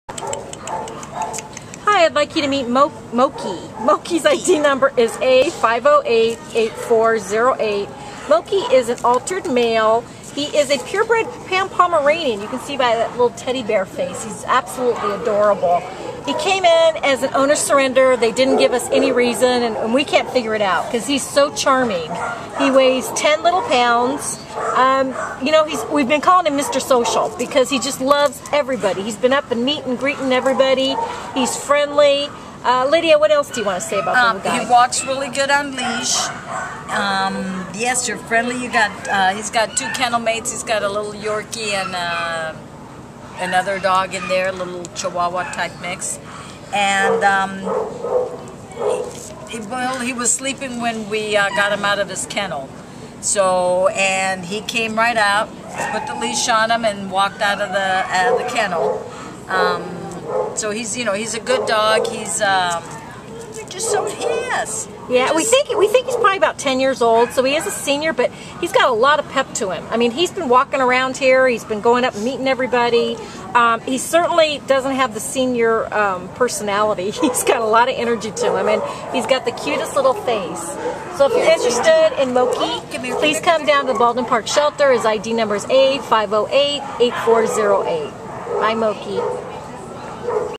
ADOPTED! A5088408 Mochi | Pomeranian
A5088408 Mochi is a cute 10-year-old carmel neutered male Pomeranian who came to the Baldwin Park Animal Care Center as an owner surrender on July 11th (no reason given). Weighing 10 lbs, Mochi is so full of personality that his handler named him Mr. Social Butterfly...he loves everyone, both people and dogs. Mochi is a true gentleman who wants to please everyone around him. He has the cute “teddy bear” face that Pomeranians are known for and a sweet disposition to match. This friendly guy is truly a great ambassador for his breed. Mochi does have alopecia, a hair loss condition that usually can be easily treated. This wonderful gentleman will make someone an awesome indoor pet and loving companion.